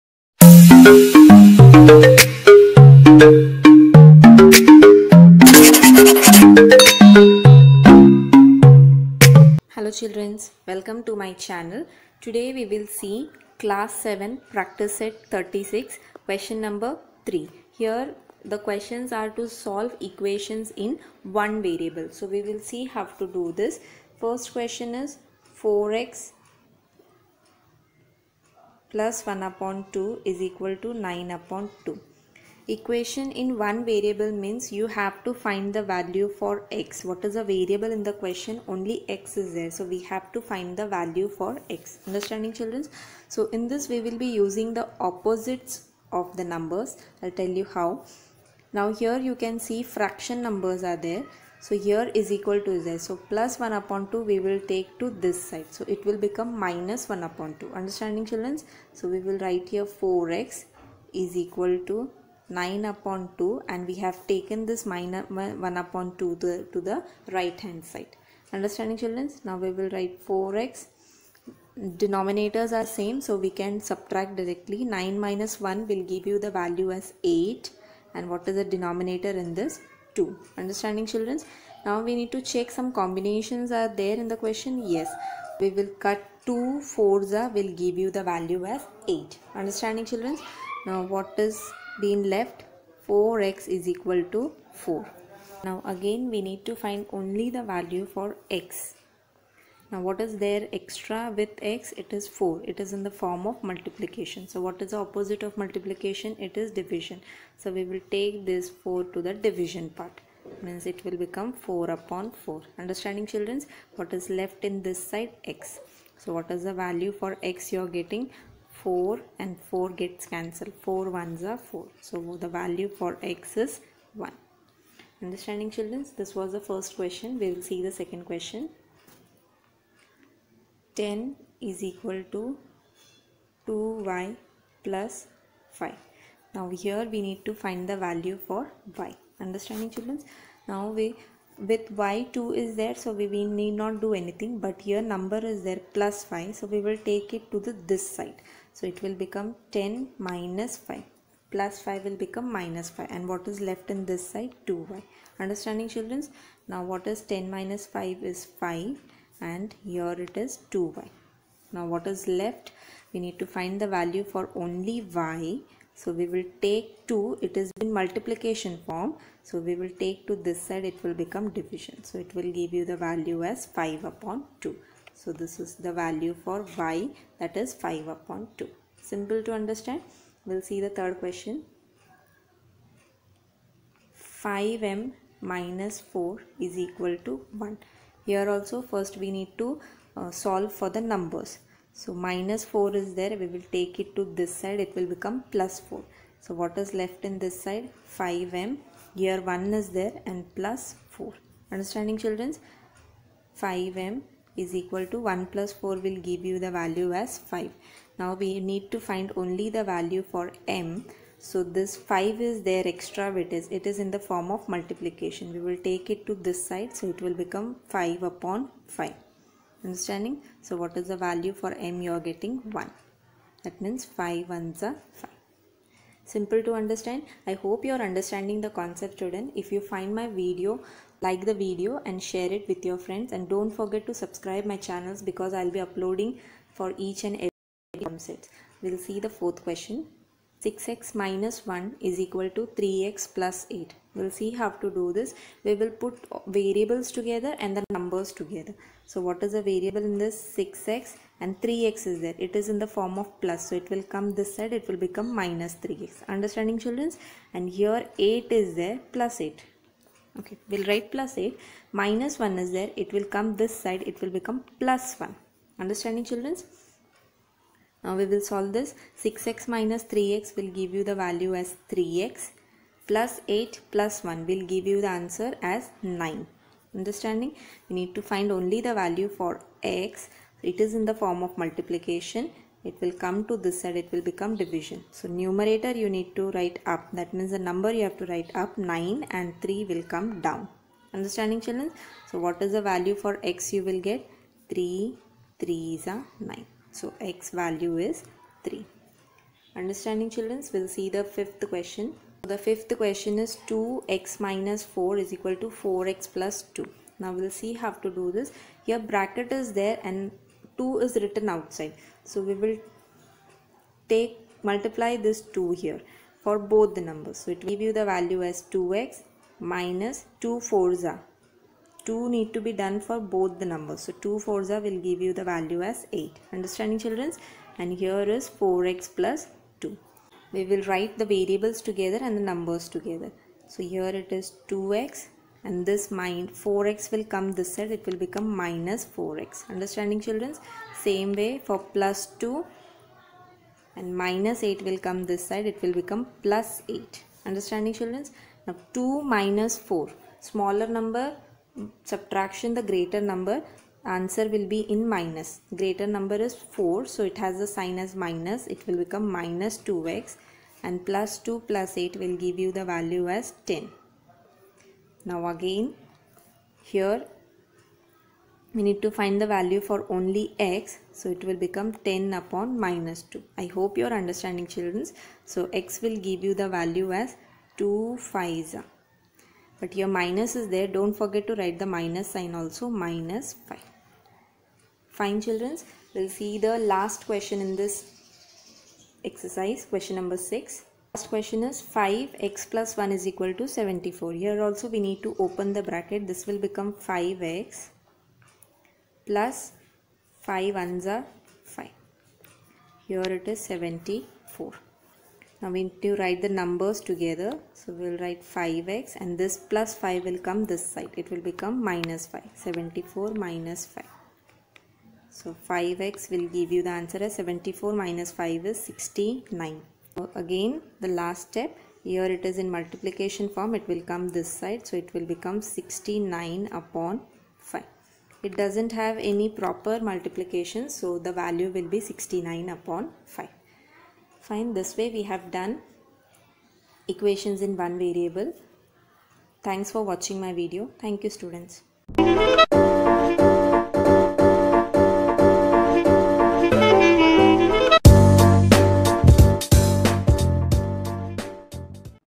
Class 7 Maths || How to find value of x || Problem set 36
Hello students,

Learn to find equation in one variable. Equation in one variable means to find the value of the variable i.e. any alphabet in the question.

This topic continues till your 10th std.